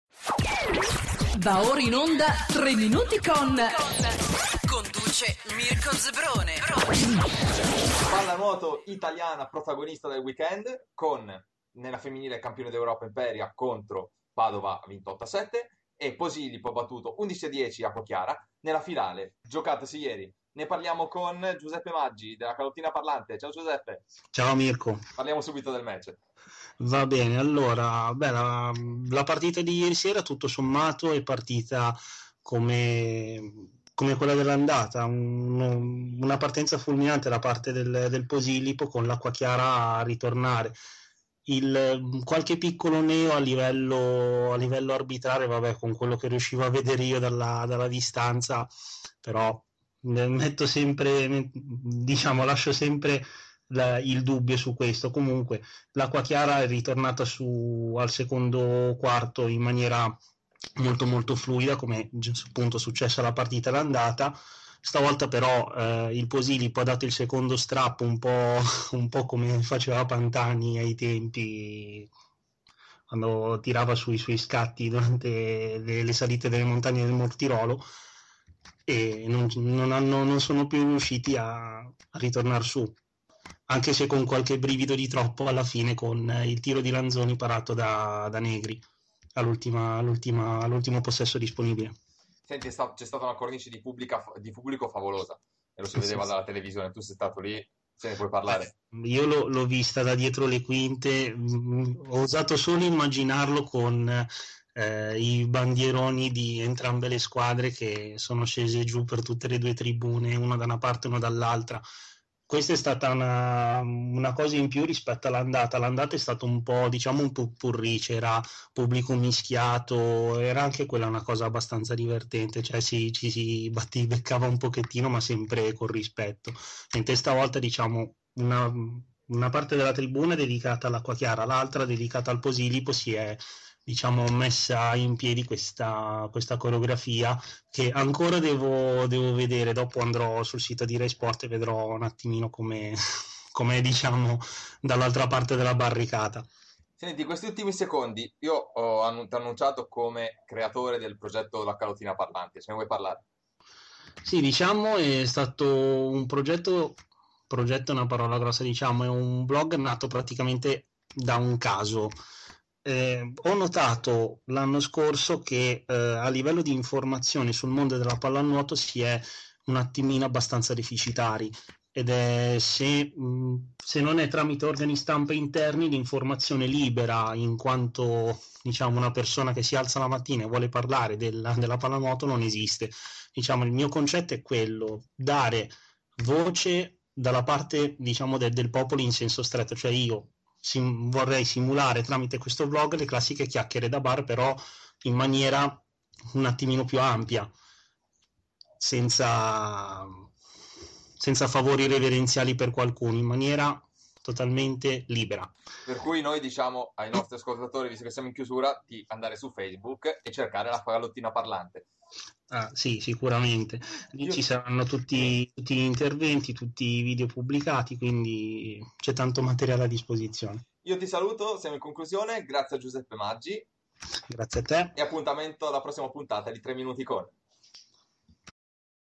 "Tre Minuti Con" Giuseppe Maggi
Ospite della settima puntata di Tre Minuti Con è Giuseppe Maggi, creatore del blog La Calottina Parlante, che ha seguito la finale di andata e di ritorno della EuroCup tra Acquachiara e Posillipo